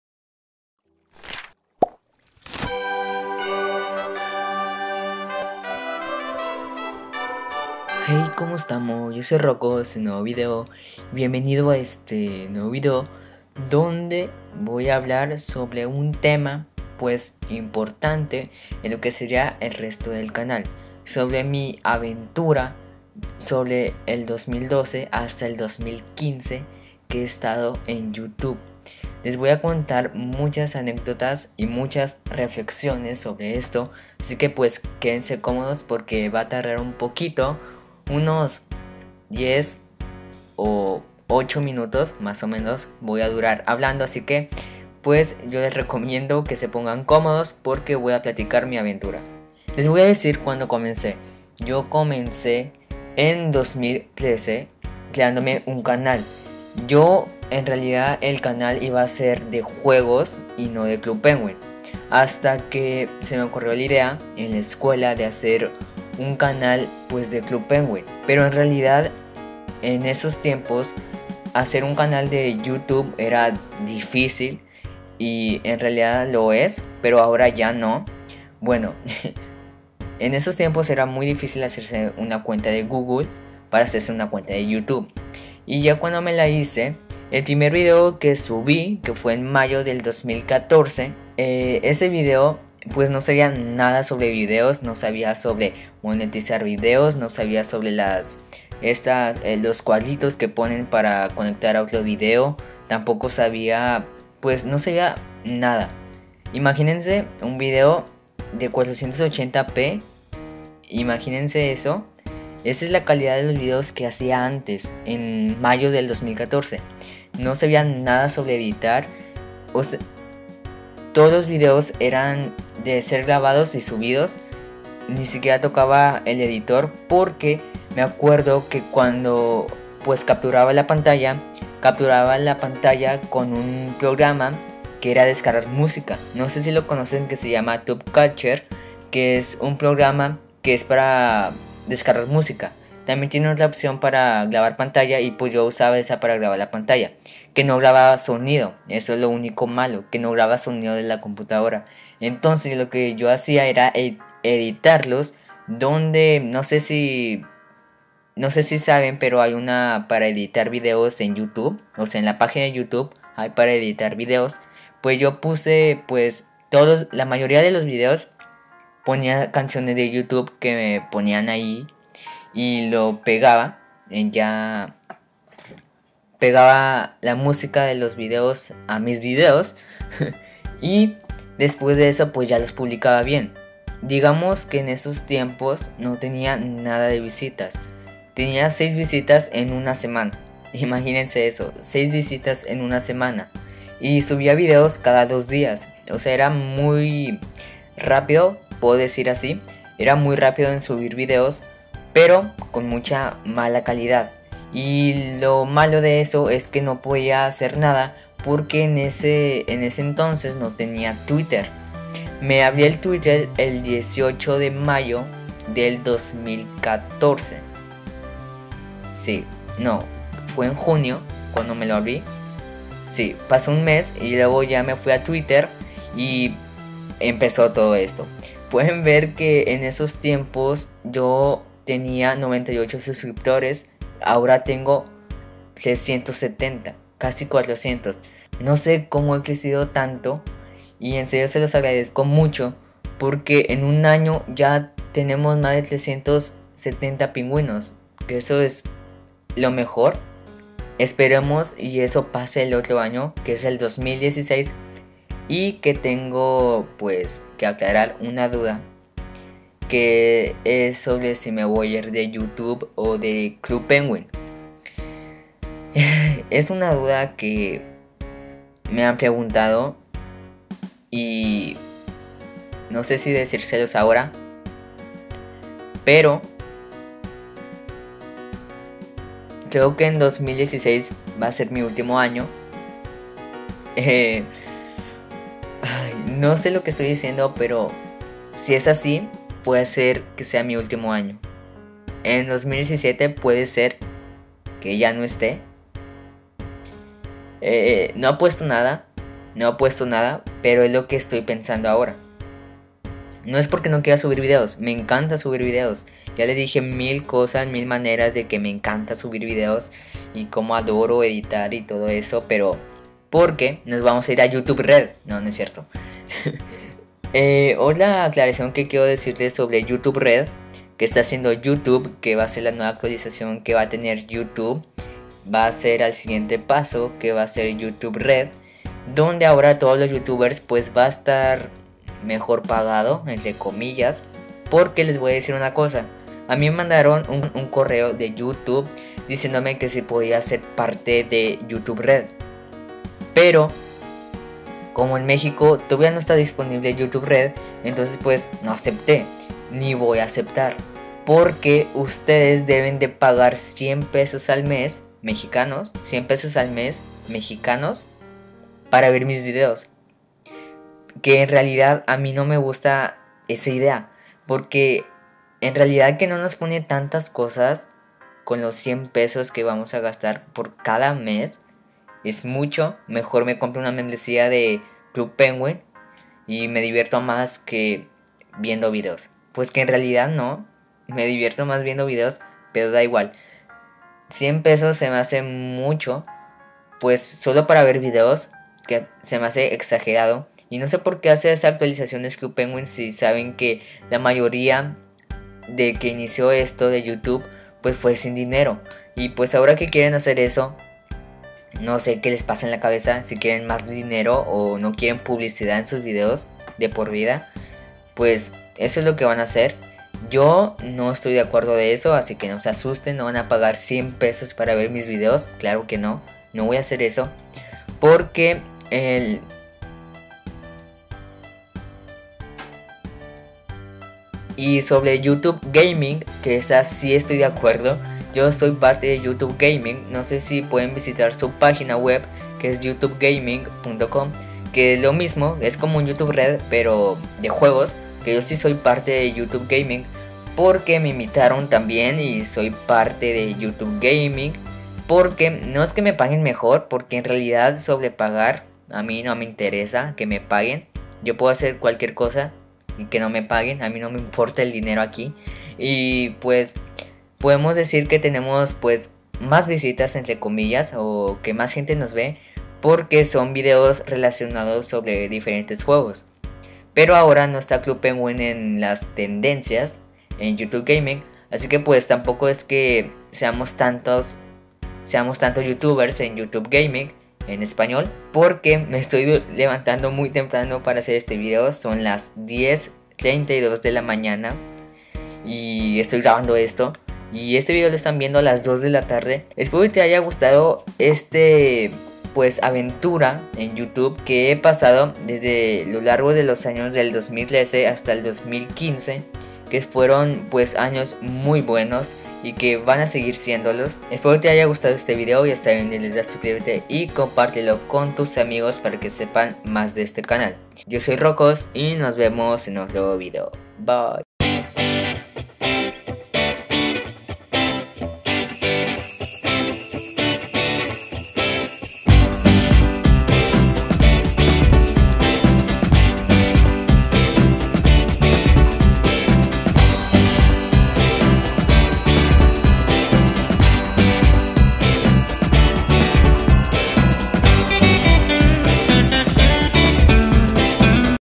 Mi aventura en YouTube - Especial de Navidad
Mi aventura de YouTube es muy interesante, de como hace 2 años llega hasta ahora, cuanto crecí, en realidad diré lo que hice y de lo que deben hacer, es sencillo. Perdonen por hablar mucho, pero soy así D: